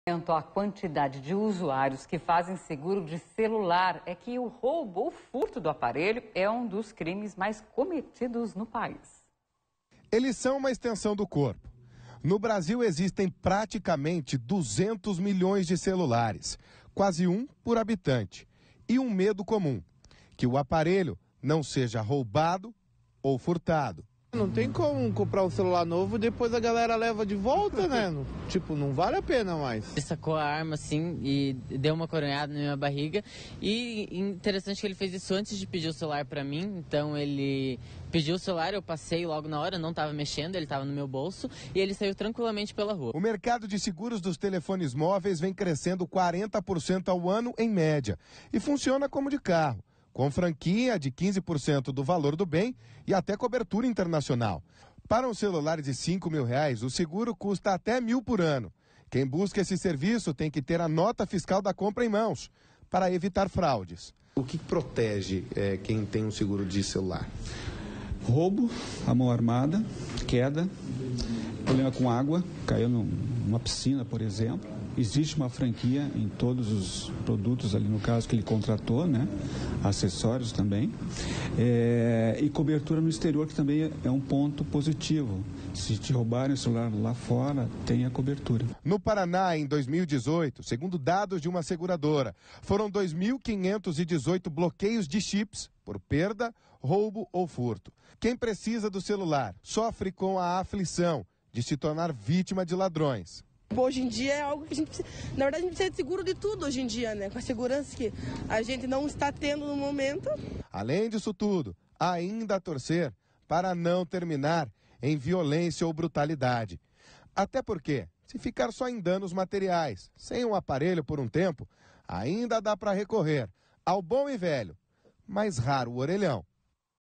Número de roubos de celulares aumenta e cria “mercado de seguros para celulares”
Omercado de seguros dos telefones móveis vêm crescendo 40% ao ano, em média. E funciona como o de carro. Com franquia de 15% do valor do bem e cobertura internacional.  Para um celular de 5 mil reais , o seguro custa 1000 reais por ano. Quem busca esse serviço tem que ter a nota fiscal da compra em mãos, para evitar fraudes.